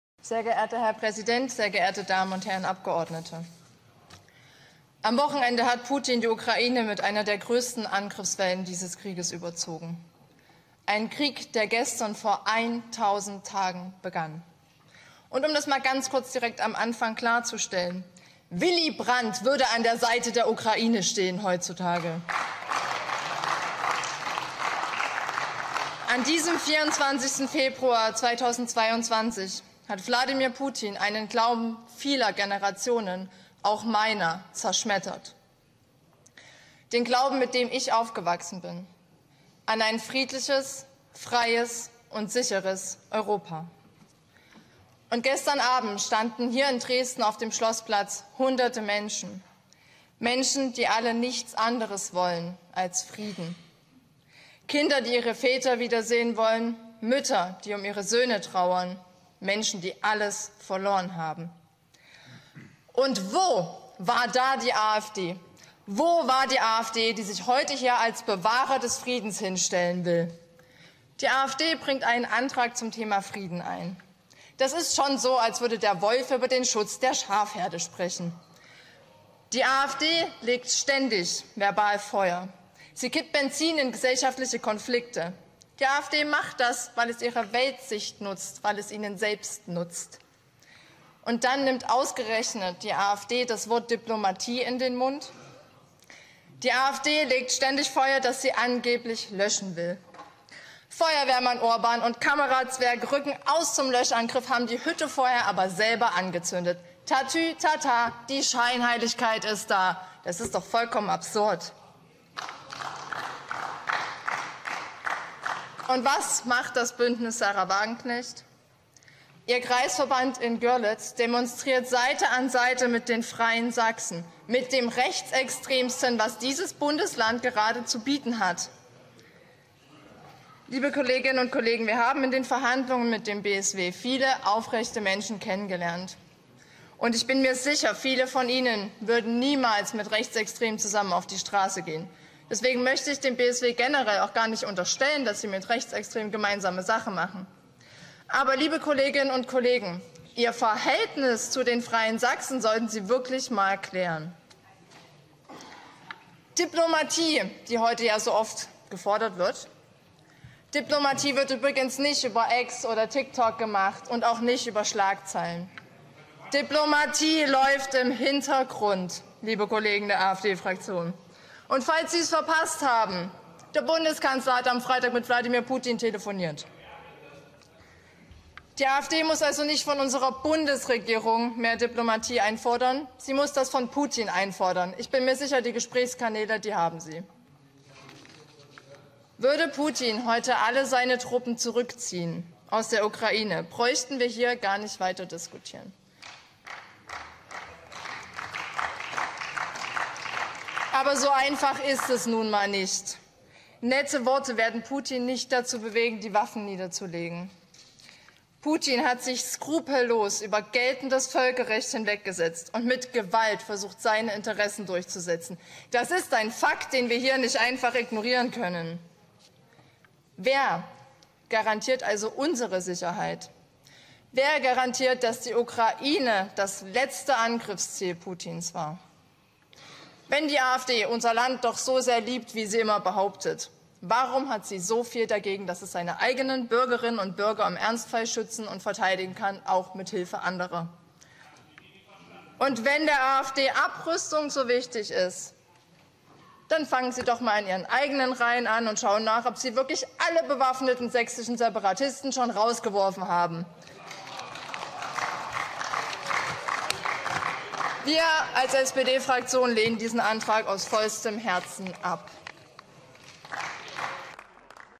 Debatte zu Raketen im Landtag
Die AfD wollte im Landtag über Raketenstandorte reden. Was die SPD-Europa-Politikerin Sophie Koch davon hält, macht sie in ihrer Rede klar.